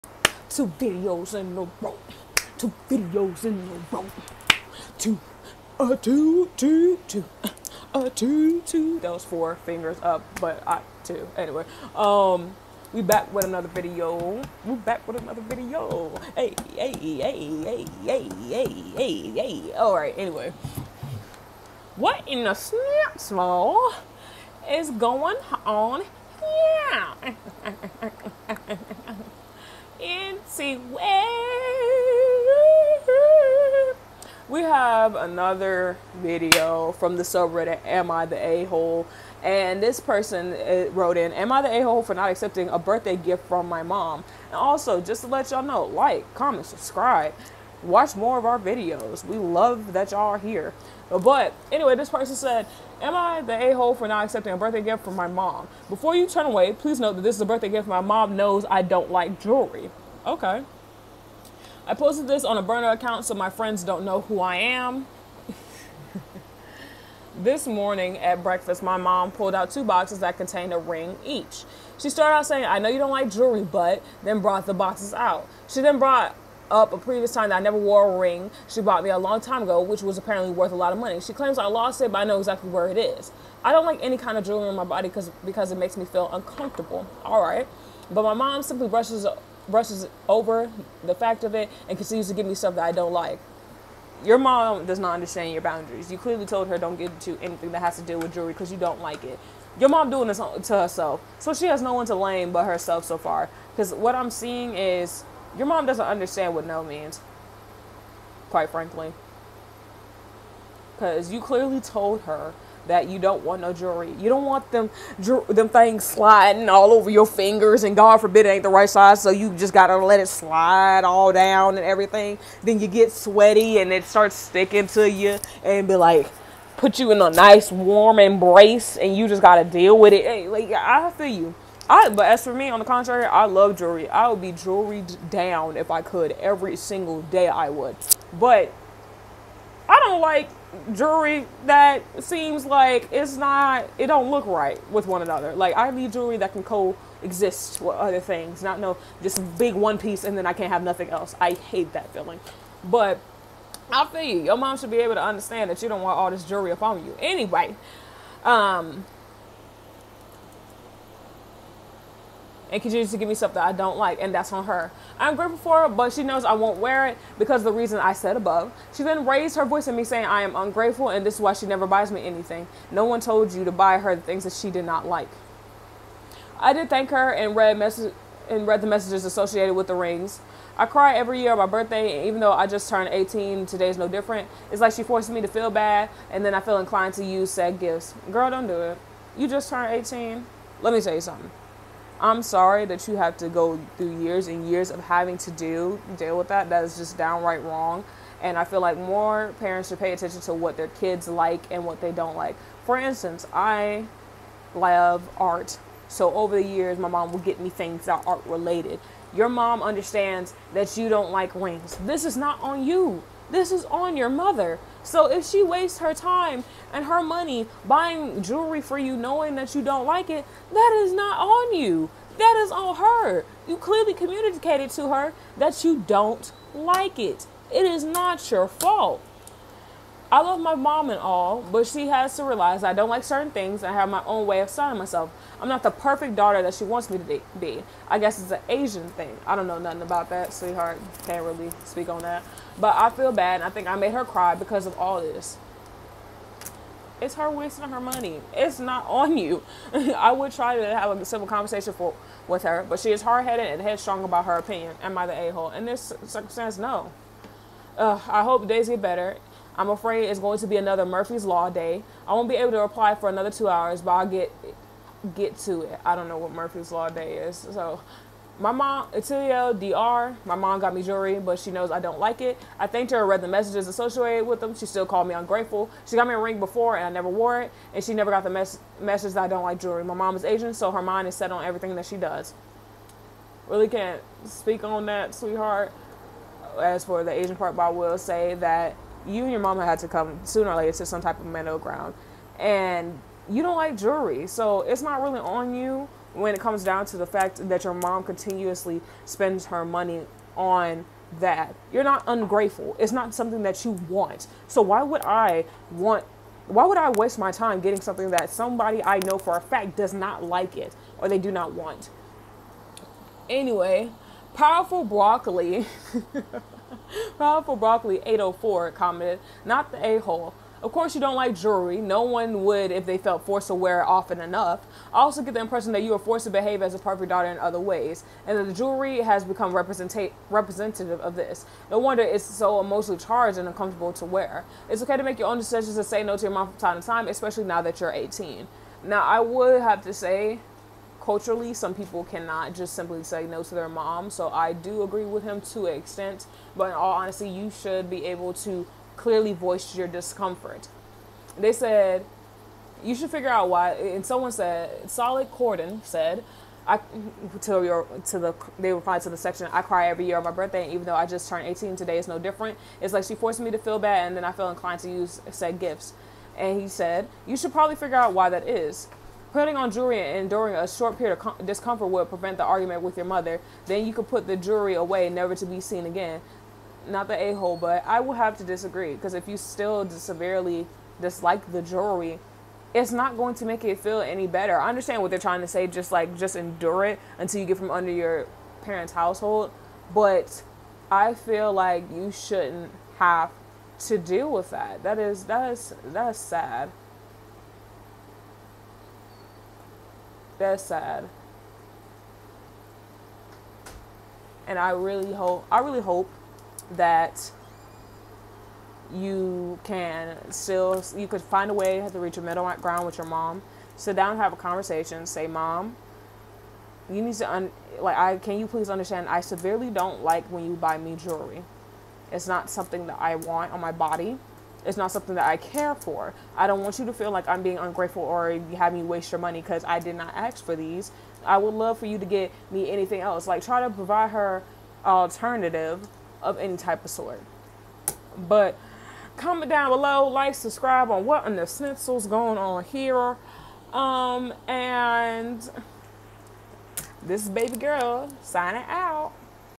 AITA for standing up to Mom
A daughter expresses her wishes to her mom but….
Watch video to find out more